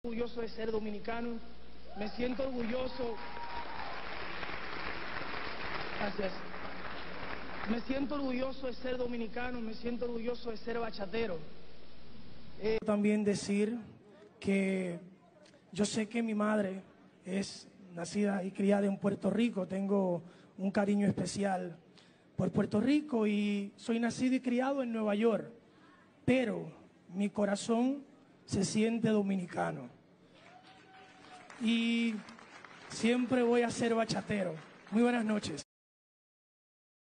ROMEO SANTOS NIEGA SER DOMINICANO EN PREMIOS SOBERANOS 2021
Luego de en ocasiones anteriores haber confirmado ser Dominicano, el cantante Romeo Santo en los Premios Soberano 2021 niega ser dominicano.